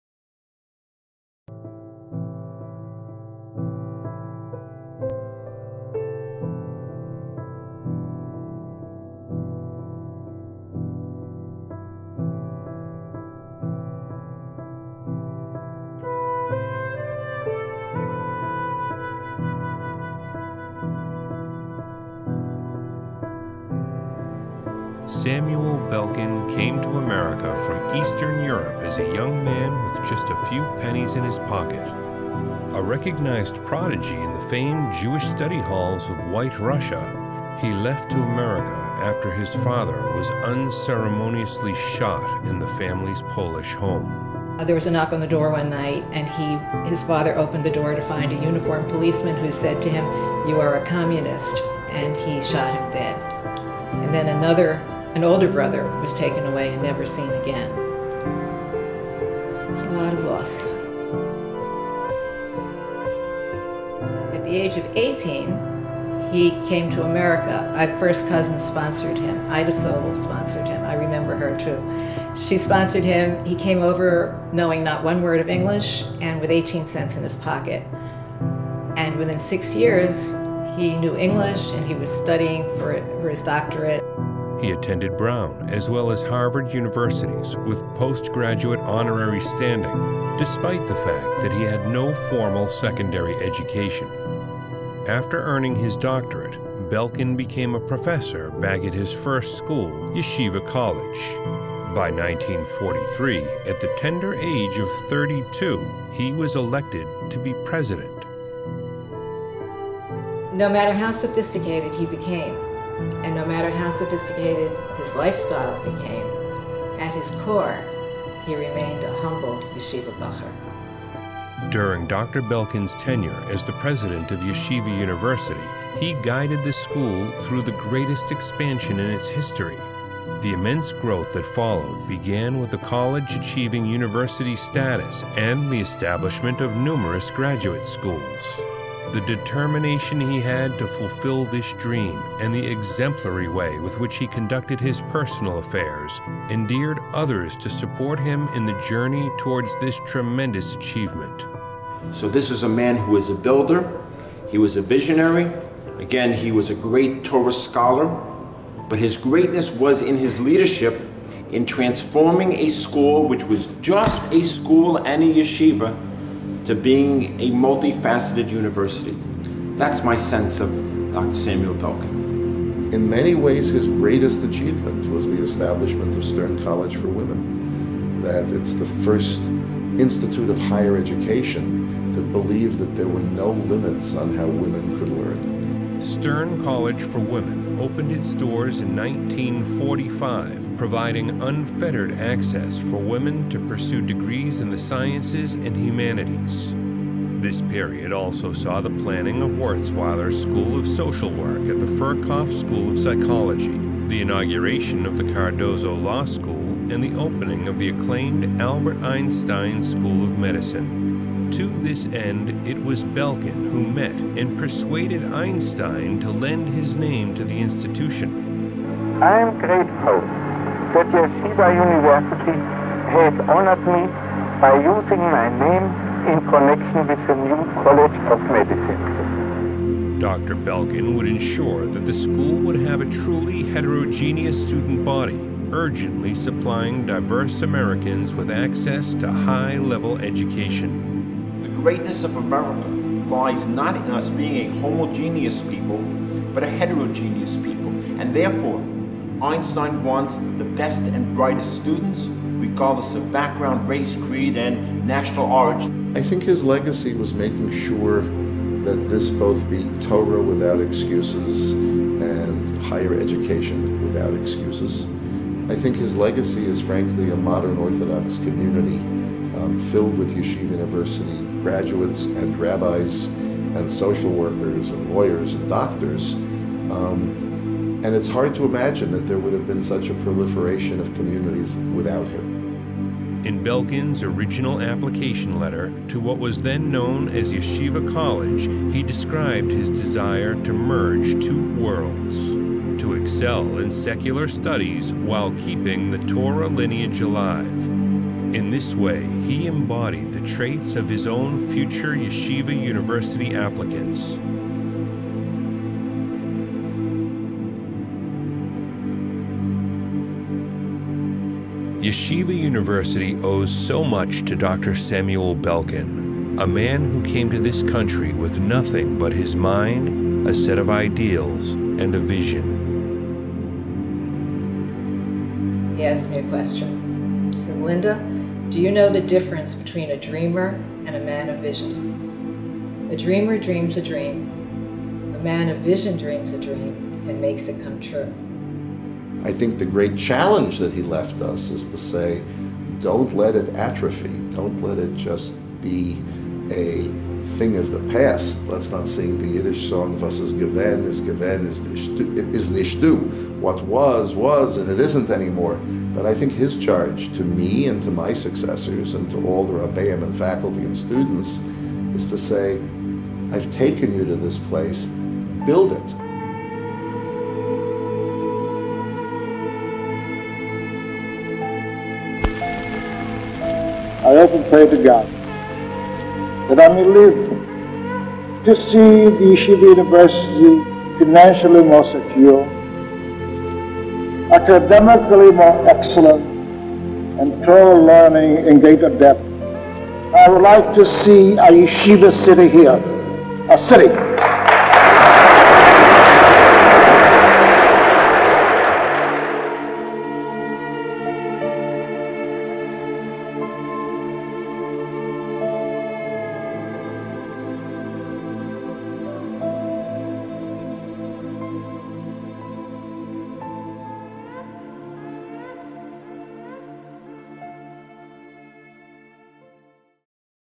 A tribute to Rabbi Dr. Samuel Belkin
The man who changed the face of orthodoxy in America. A tribute to Rabbi Dr. Samuel Belkin